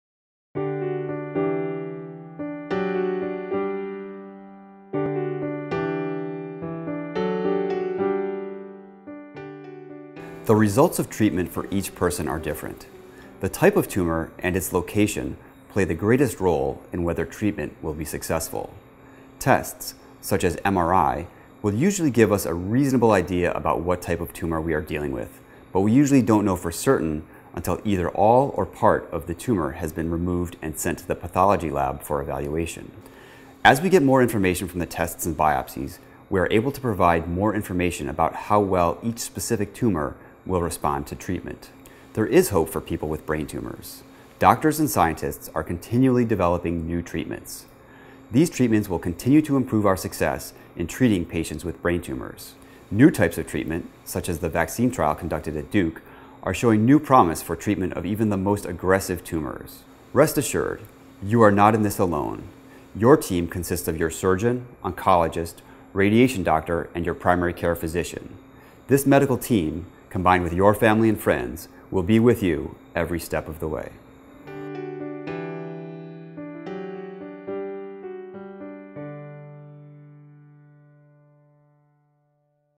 What Are My Chances of Surviving Brain Cancer: Dr Seth Joseffer
Dr  Seth Joseffer discusses the advances in brain tumor care, and their probablilty of effectiveness.